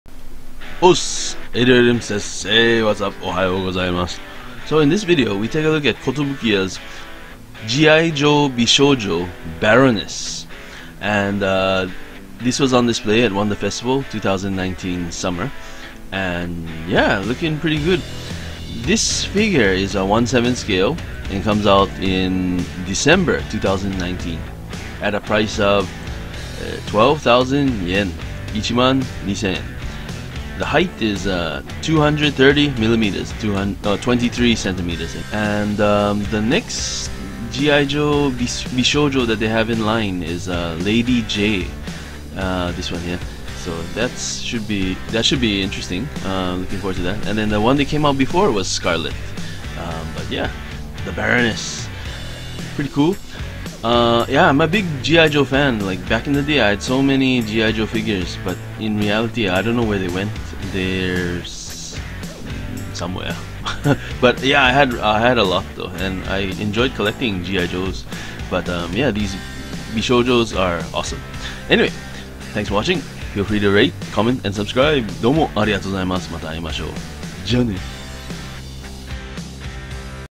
WF2019S Kotobukiya - Baroness - G.I.JOE Bishoujo コトブキヤ - バロネス G.I.JOE 美少女
A look at Kotobukiya's G.I.JOE Bishoujo - Baroness.   This figure was on display at Wonder Festival 2019 Summer.